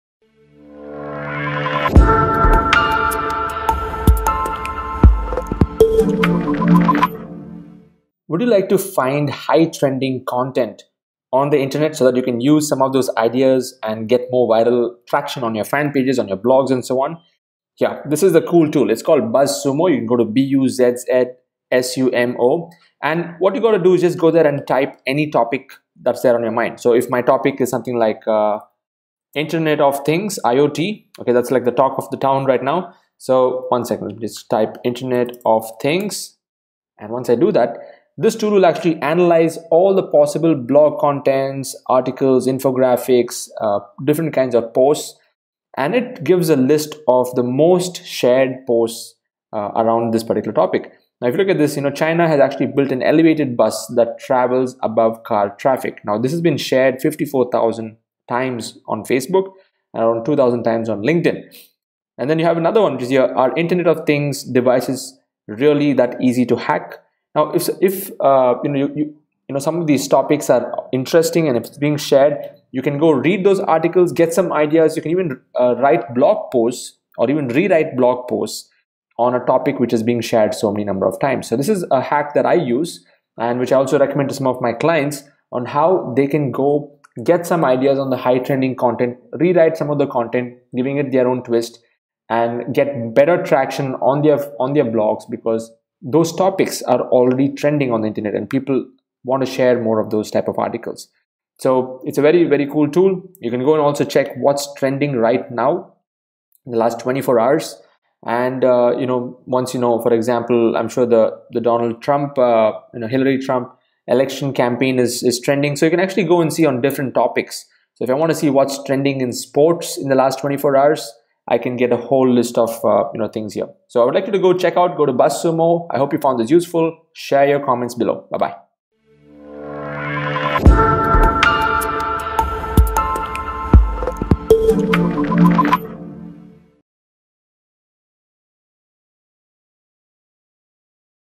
Use BUZZSUMO To Find Super Viral Content Online
- BuzzSumo is a research and monitoring tool. The functionality is broken down as follows: Find content that is most shared on social media channels – you can enter your own domain or another domain, and see what has been shared socially. Find influencers related to particular topics